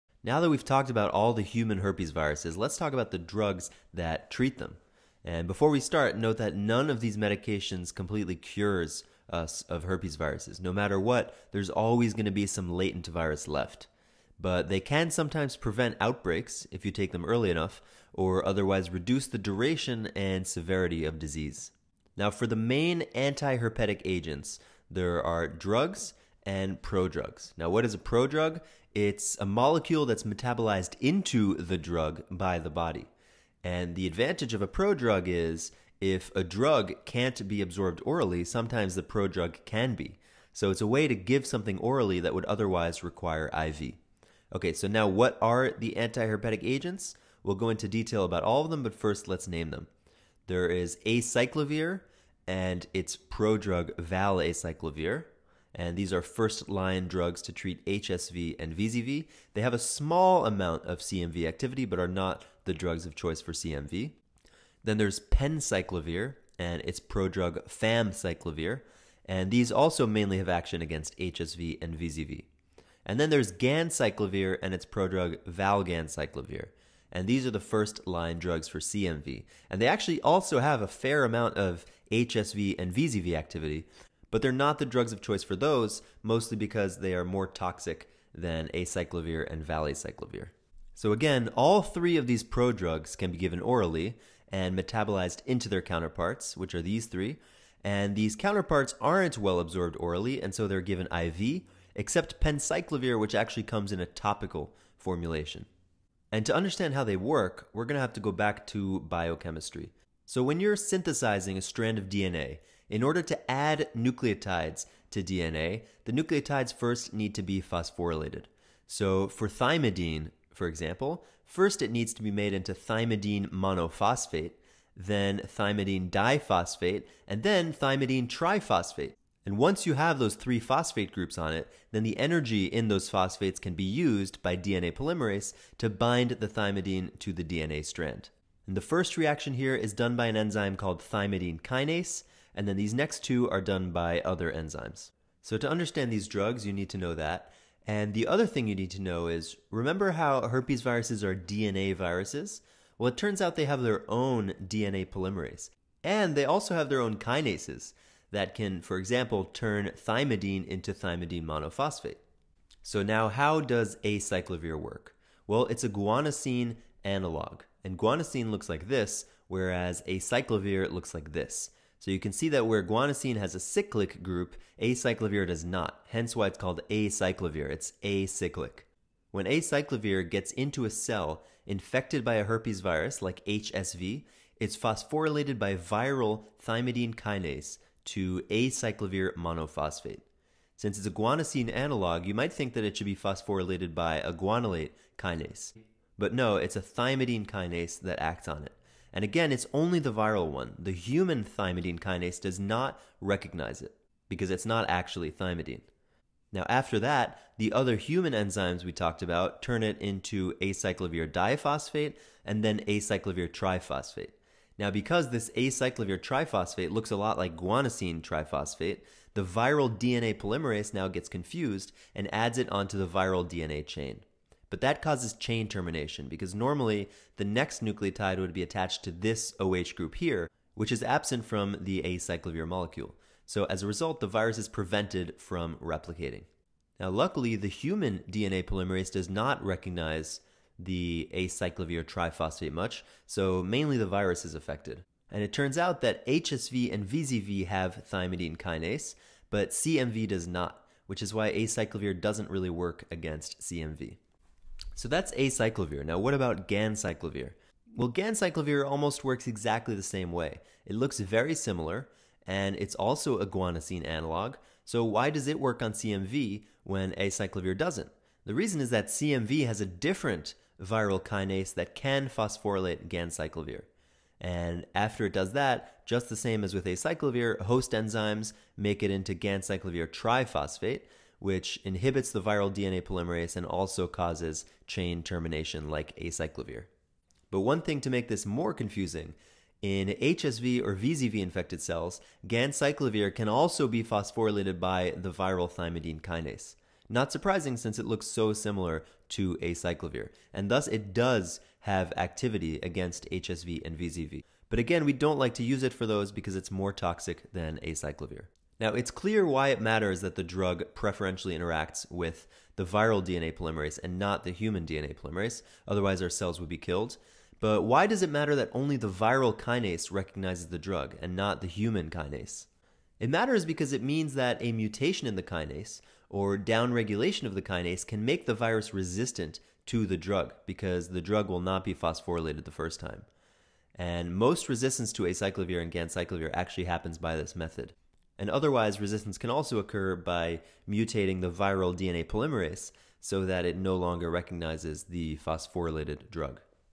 Anti-Herpetic Drugs - How They Work
Medications to treat herpes viruses include acyclovir/valacyclovir, penciclovir/famciclovier, and ganciclovir/valganciclovir. These medications are nucleoside analogues that are preferentially recognized by viral kinases.